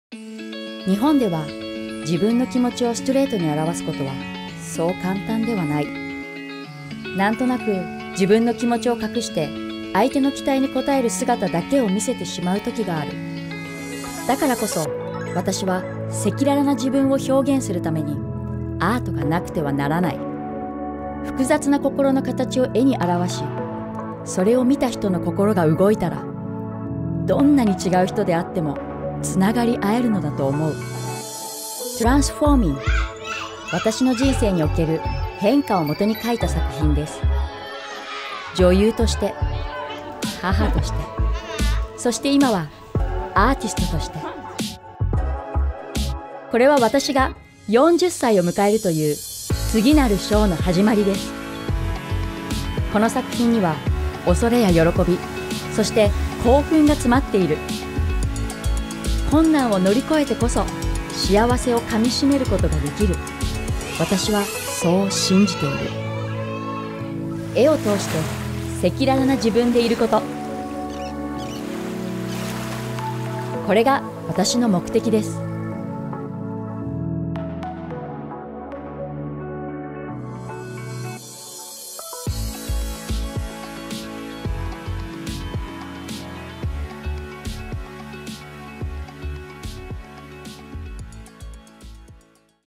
2022 UOB Painting of the Year - Nikki Yamaguchi (Artist Film)
"Through art, I can be who I really am." - Nikki Yamaguchi, 2022 UOB Painting of the Year (Singapore), Bronze Award, Emerging Artist Category.